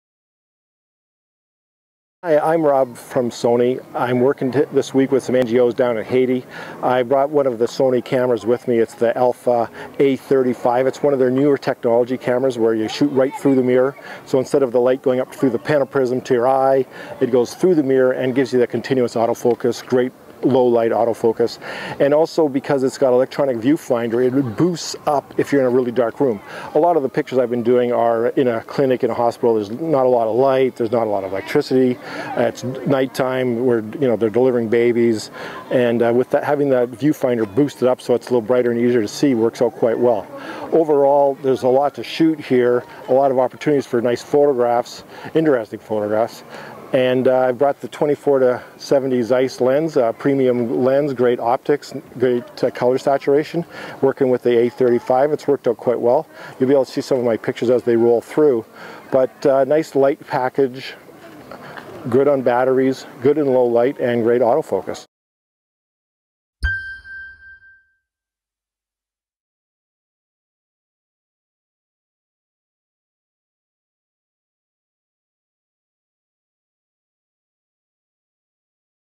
Sony Alpha 35 in Haiti
The Sony Alpha A35 uses the latest Revolutionary Translucent Mirror Technology to help the photographer make better images. Sony Digital Imaging Trainer Rob Skeoch, had a chance to test the camera in the field while working in rural Haiti.